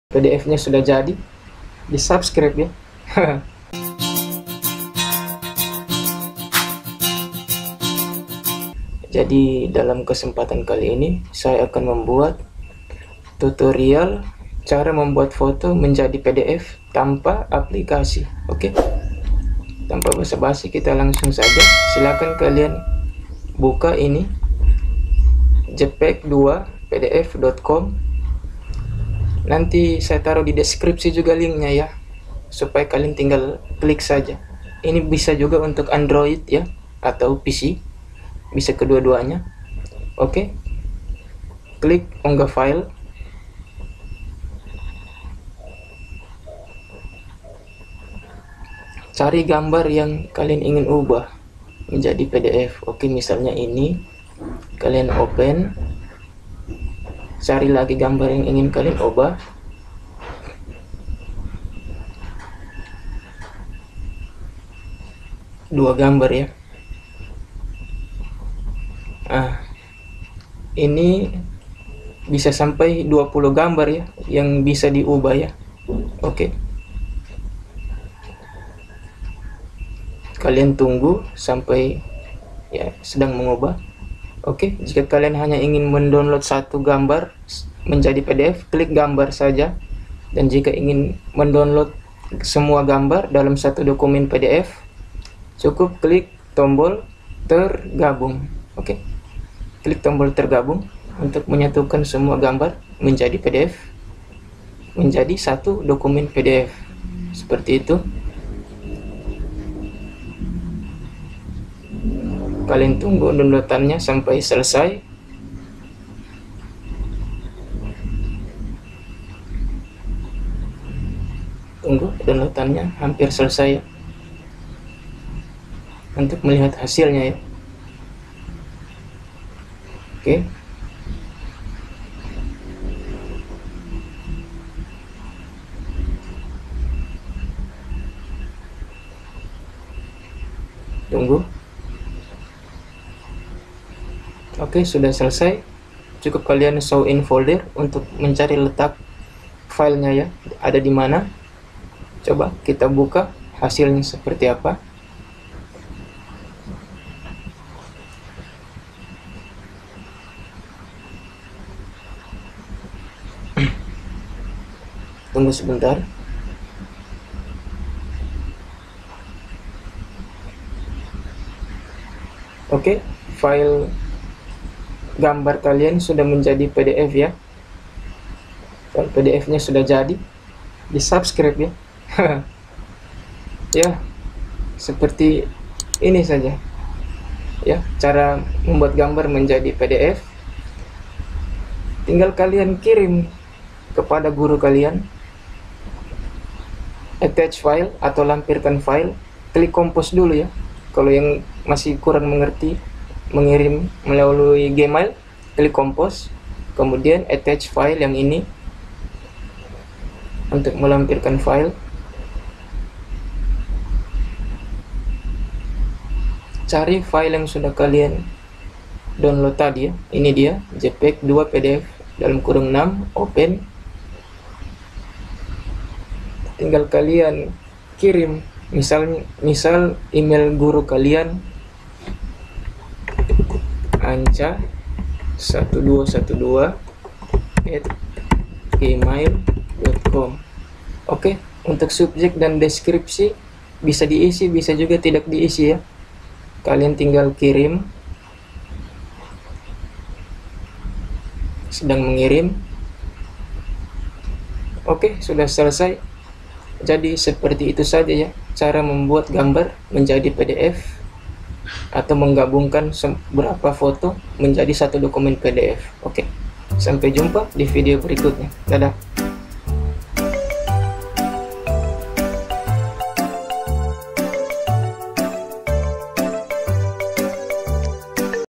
Tutorial Cara Mengubah Foto Menjadi Pdf Tanpa Aplikasi | Bisa Untuk Android Dan Laptop
Cara Membuat Atau Mengubah Foto Menjadi Pdf. Tutorial Cara Mengubah Foto Menjadi Pdf Tanpa Aplikasi | Bisa Untuk Android Dan Laptop

Jadi Pada Kesempatan Kali Ini Saya Membuat Cara Mengubah Foto Menjadi Pdf Tanpa Aplikasi Di Komputer, Laptop Dan Android, Semoga Video Ini Bermanfaat.